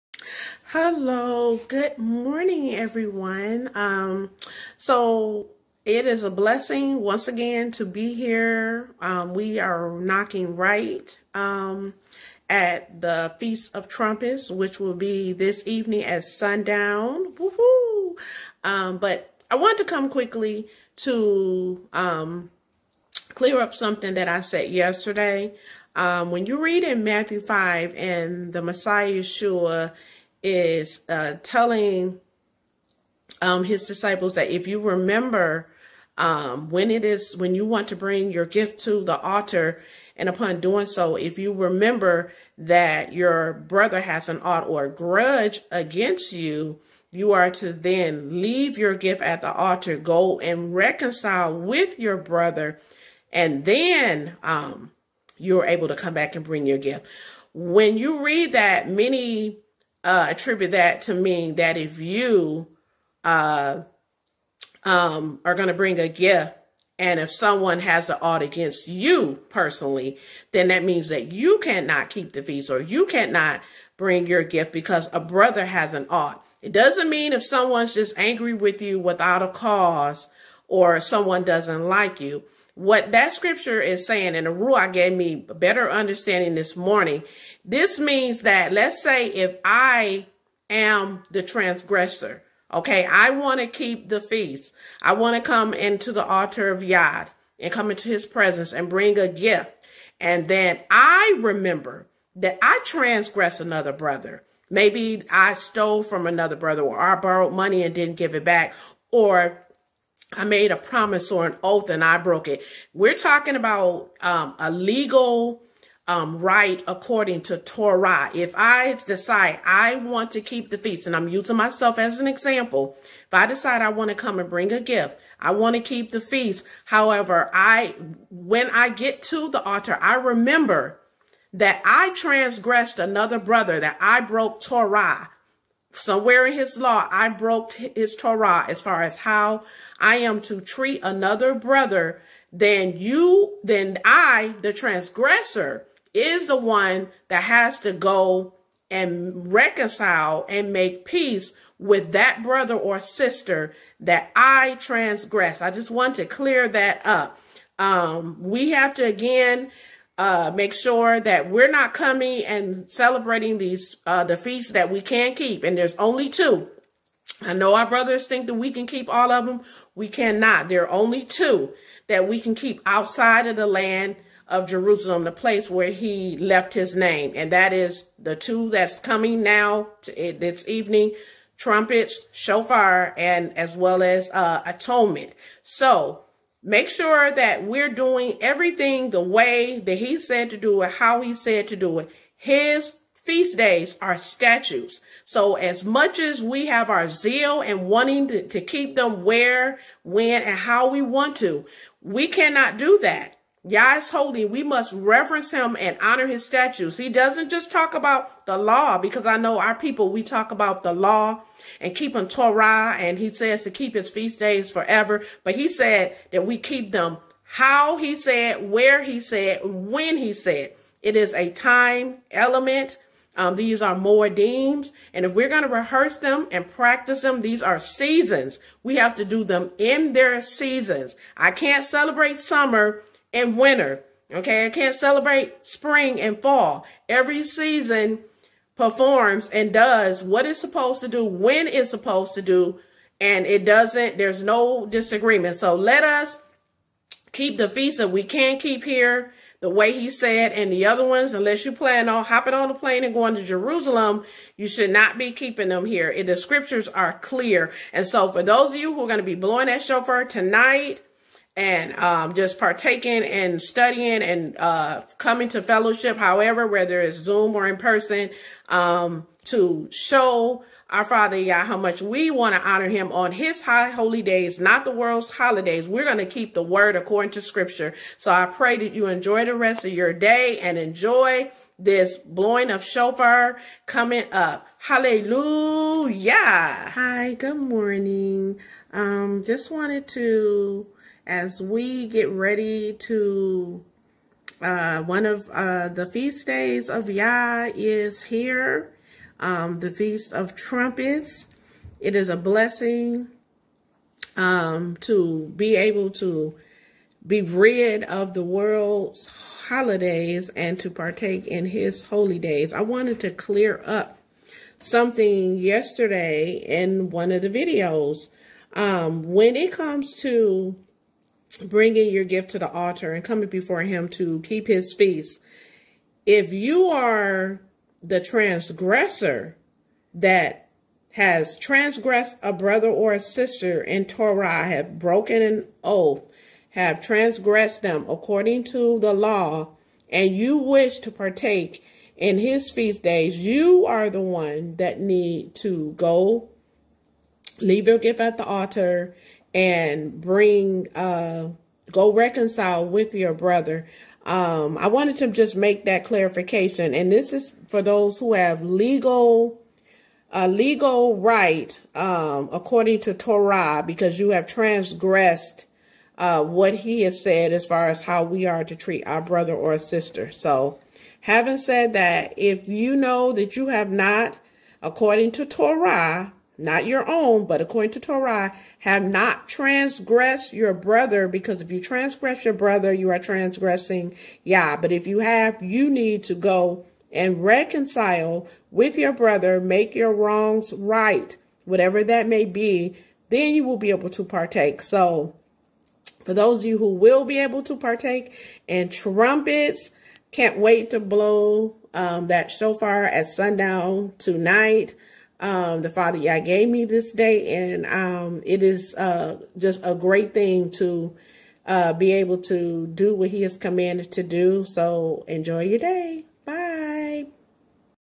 Message!!! | Feast of Trumpets is Here!!! | Clearing Up Matthew 5:23-24
FEAST OF TRUMPETS is from Friday, September 18th sundown to Saturday, September 19th sundown. In light of my last video message regarding the feasts and bringing gifts to the alter of Yah during the feasts, I would like to clarify and expand on a point that I was making. Shalom!